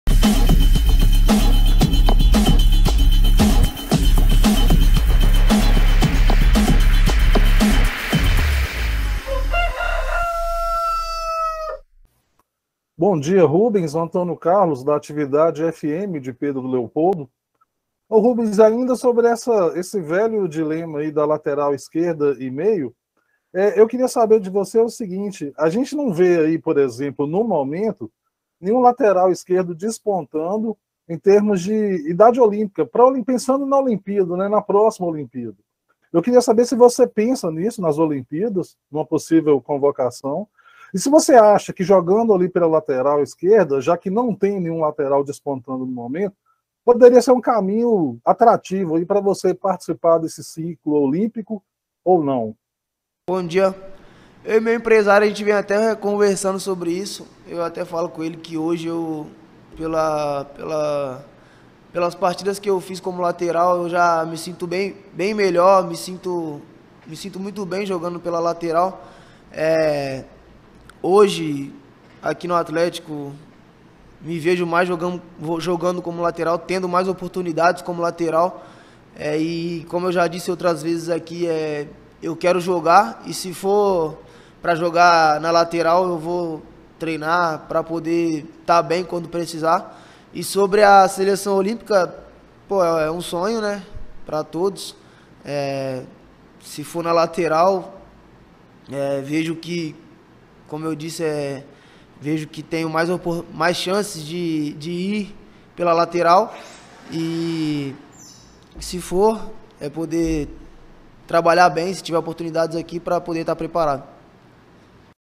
Galo Análises nas coletivas: Rubens
Em mais uma participação nas coletivas do Atlético Mineiro, perguntei ao Rubens sobre as possibilidades de participar das próximas Olimpíadas jogando como lateral esquerdo. Confira!.